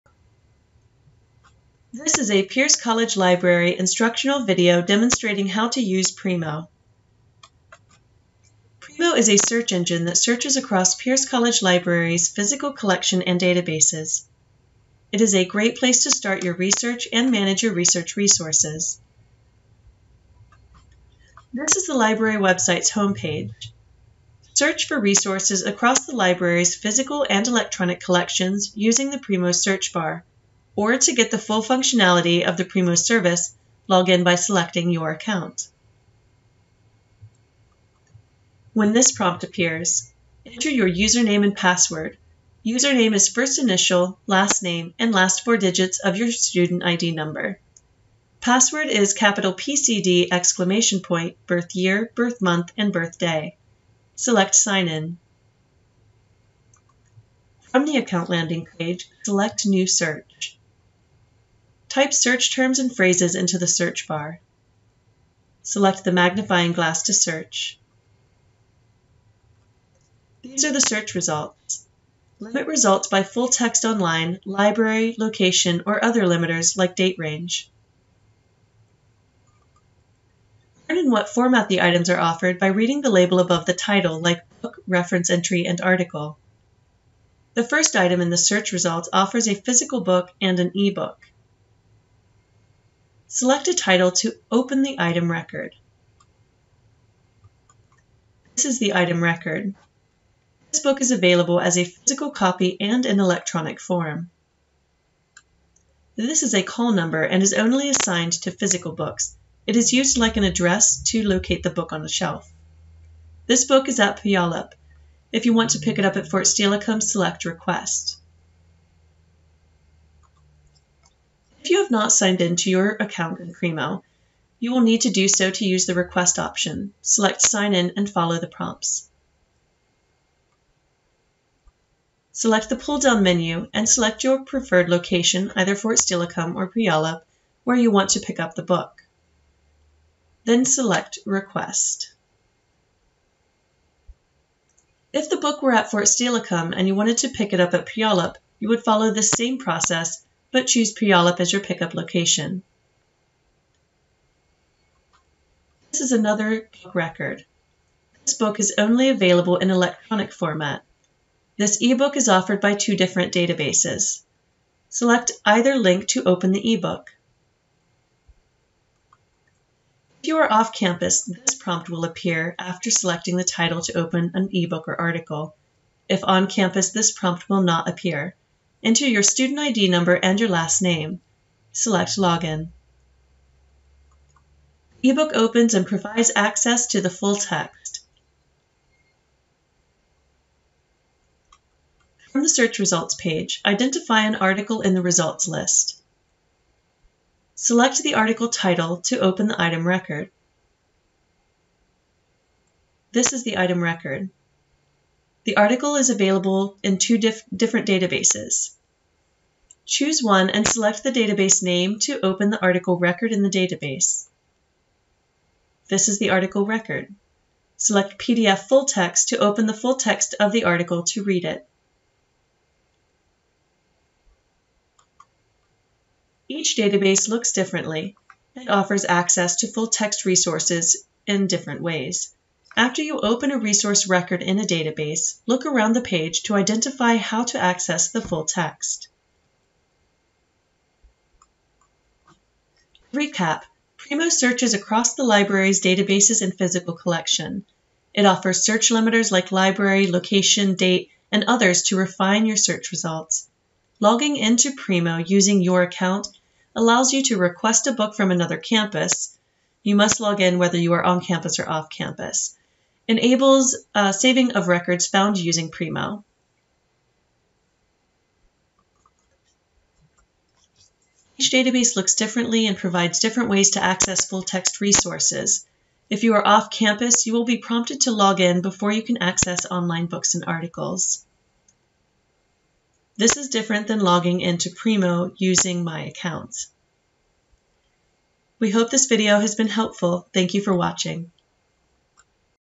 Use Primo to find resources
Outcomes:
Sign-in to Primo
Distinguish between source types
Use limiters, like location
Request a book
Open online resources, like a book and an article